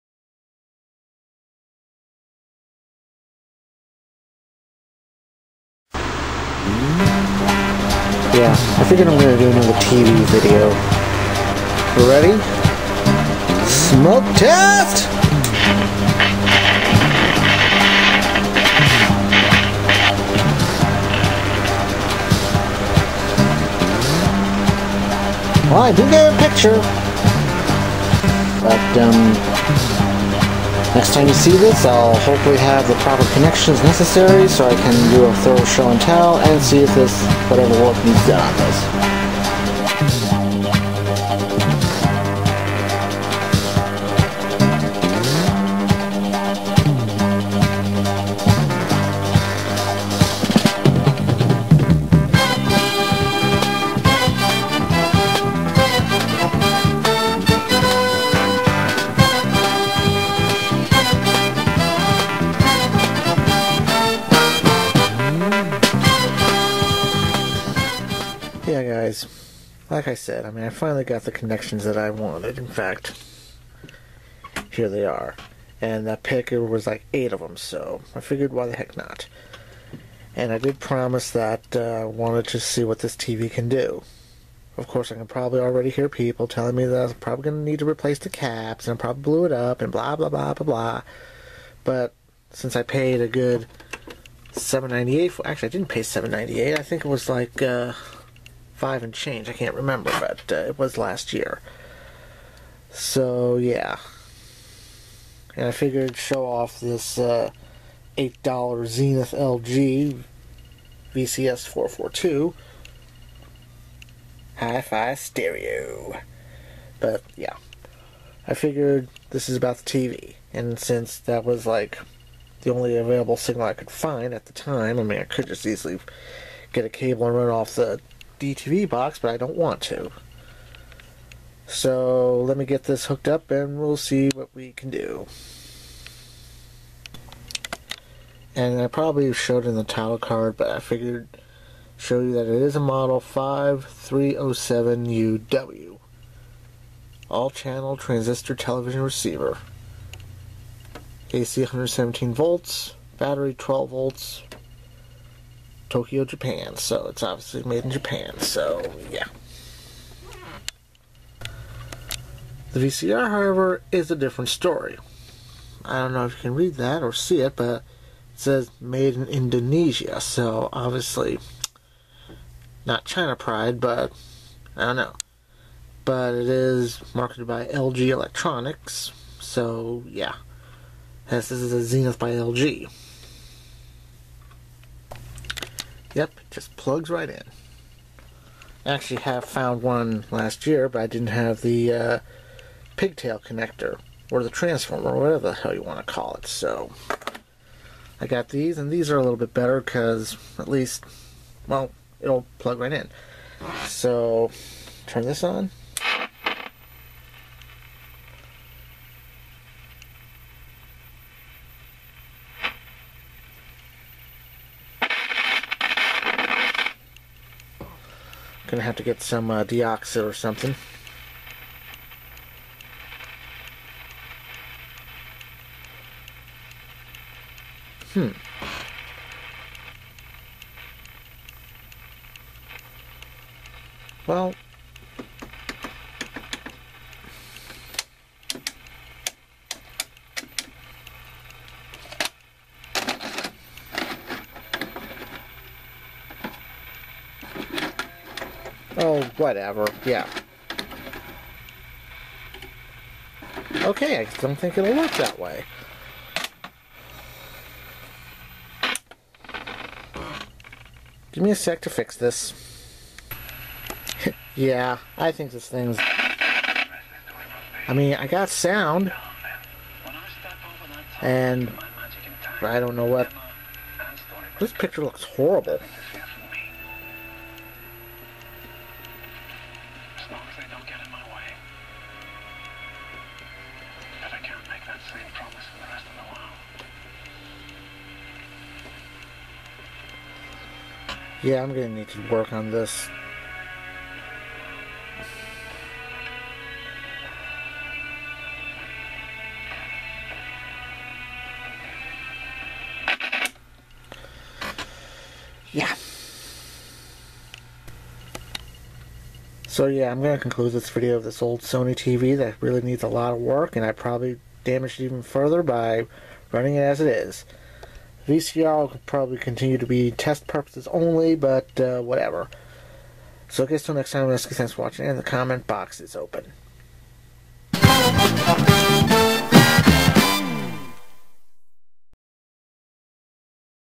Eskie's Vlog 102515: Revisiting the Sony TV
A revisit of this old Sony TV from the 60's or so that I was promised last year, but getting to it now. The VCR, it isn't that special.